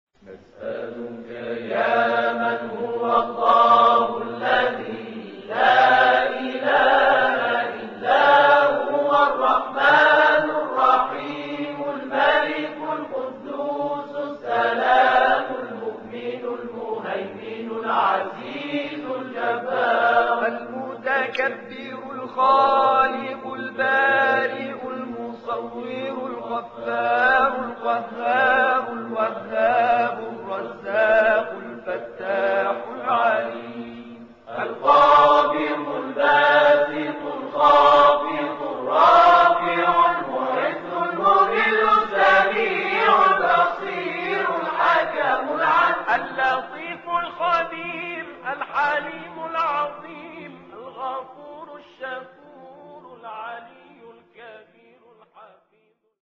quran exhibition with Iranian art of Calligraphy at the National Assembley of pakistan
quran exhibition with Iranian art of Calligraphy was displayed at the NationalAssembley of our brotherly country of Pakistan on April 18th, 2023. It was an auspicious occasion both On the Holy night of Revelation of Quran & Pakistan lunar NationalDay. It was supported and inaugurated by H.E. Raja Pervez Asharf, Honorable Speaker of Pakistan National Assembly.